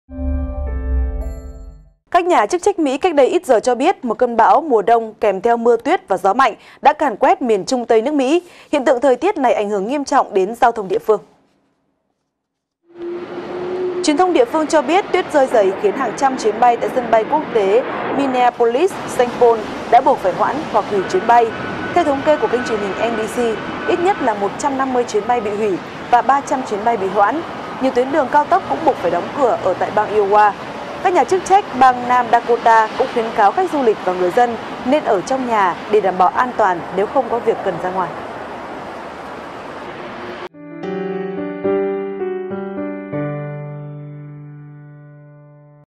VTC Tin Mới
VTC1 | Một cơn bão mùa đông kèm theo mưa tuyết và gió mạnh đã càn quét Trung Tây Mỹ. Hiện tượng thời tiết bất thường này đã ảnh hưởng nghiêm trọng tới đời sống cư dân địa phương.